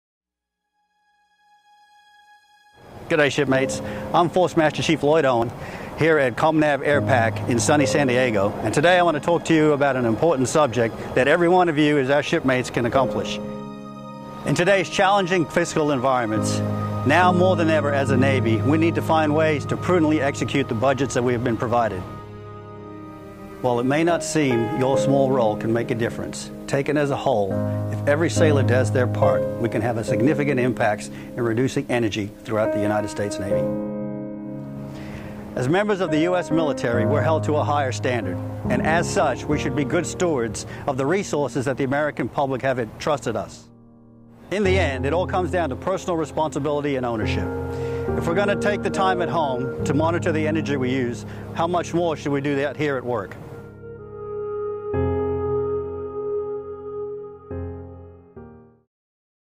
FORCE MASTER CHIEF LLOYD-OWEN TALKS ENERGY CONSERVATION
Master Chief William Lloyd-Owen, Force Master Chief for Commander, Naval Air Force, Pacific, speaks to Sailors about the importance of energy conservation in the U.S. Navy.